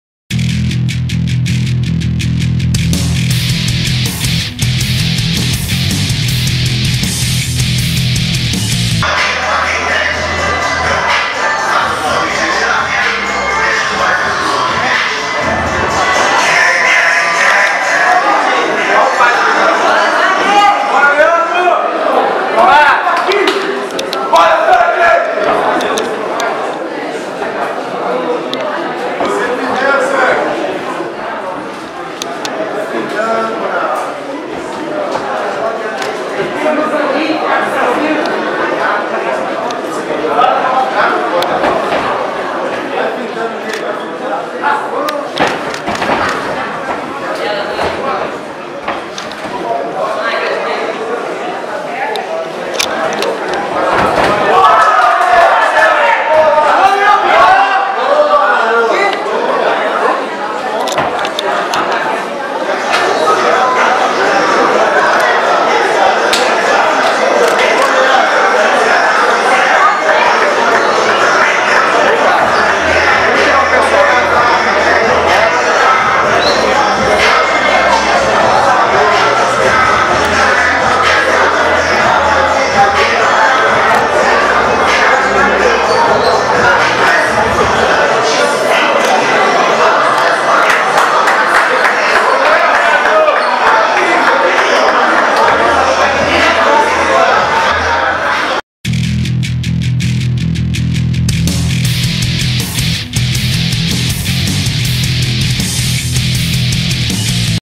Leandro vs Allan - Xtreme Grappling Championship 2
Leandro The Punisher (Team Borracha) venceu Allan (Lobo Jiu-Jitsu).